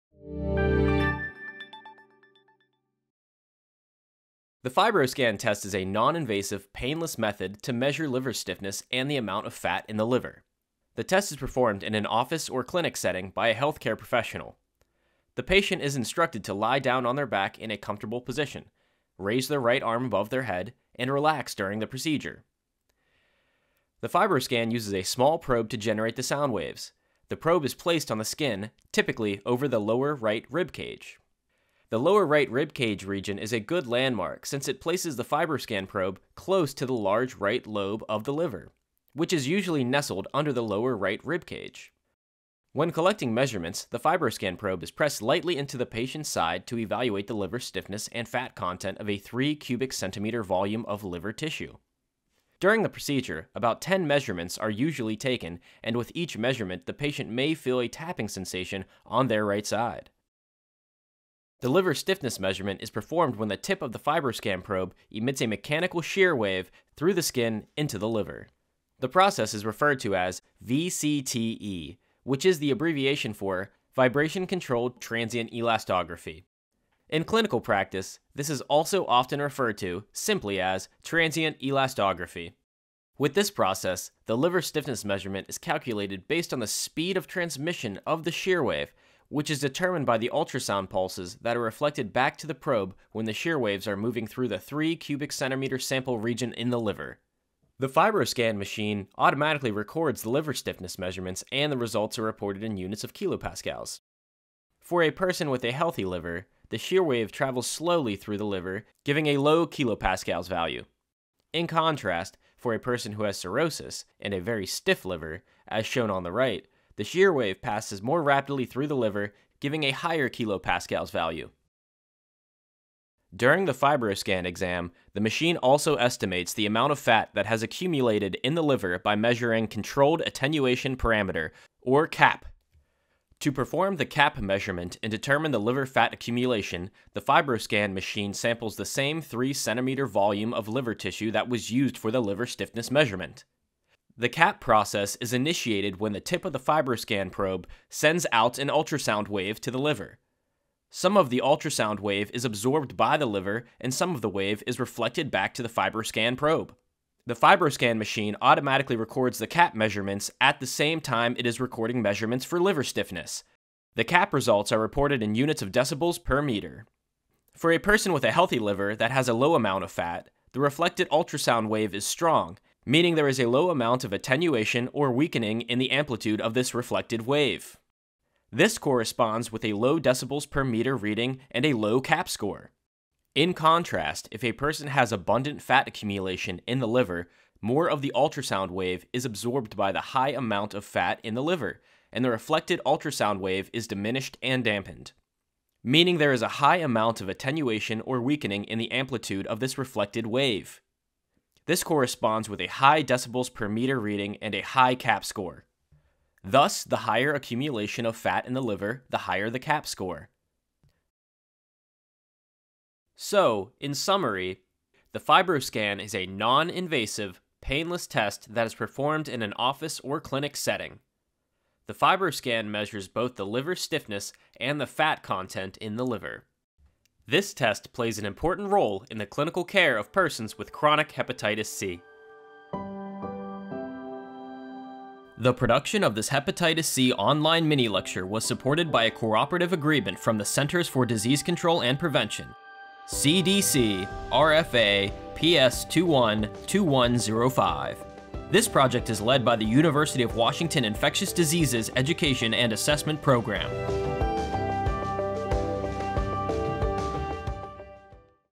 FibroScan: A Clinician's Guide
The FibroScan test is non-invasive, painless method to measure liver stiffness and the amount of fat in the liver. Dr. Nina Kim, Hepatitis C Online Associate Editor and University of Washington Professor of Medicine, reviews how to perform and interpret a Liver Stiffness Measurement (LSM) and a Controlled Attenuation Parameter (CAP). This test plays an important role in the clinical care of persons with chronic hepatitis C.